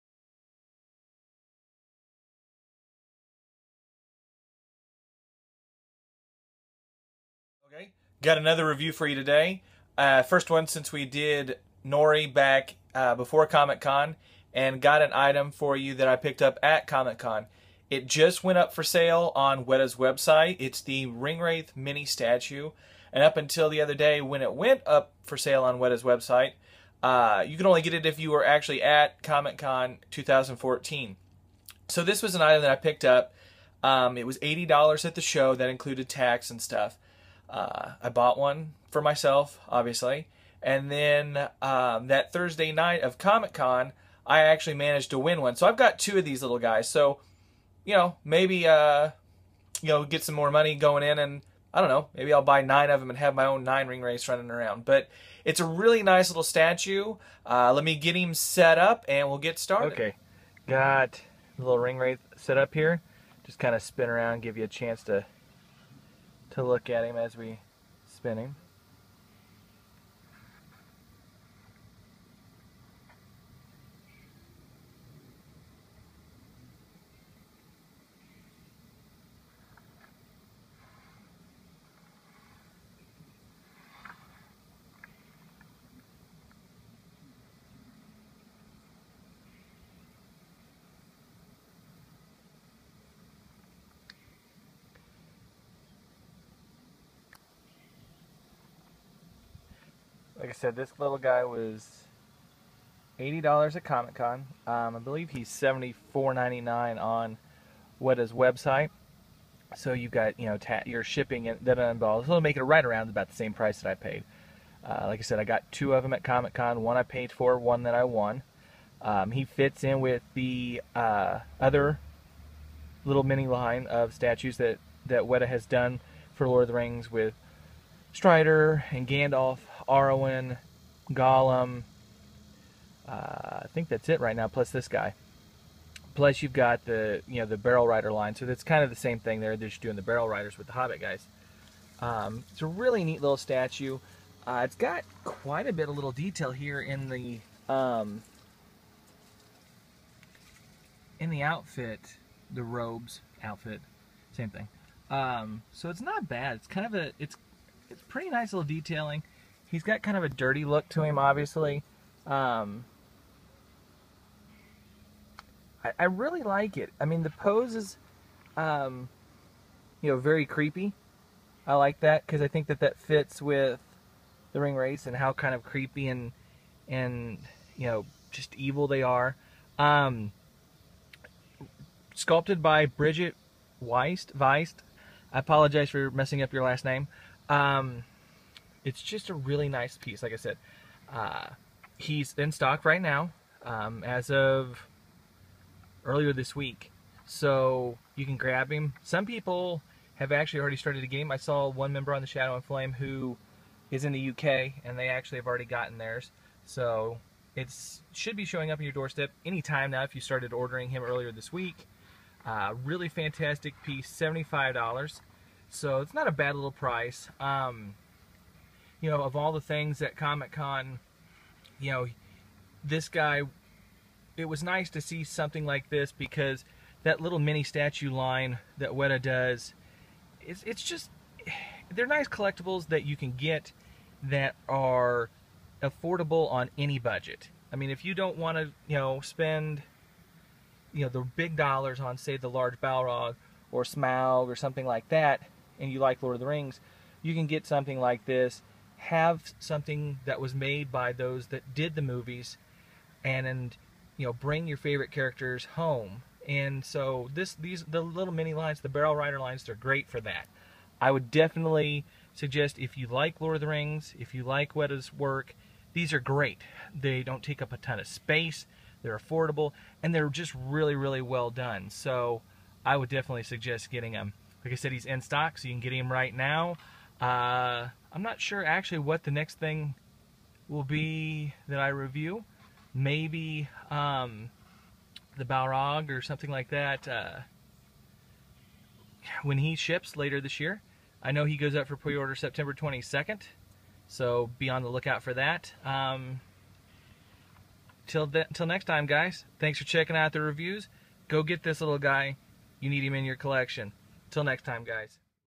Weta Workshop's Mini-Ringwraith Statue
A short review of the Mini-Ringwraith Statue by Weta Workshop for The Lord of the Rings.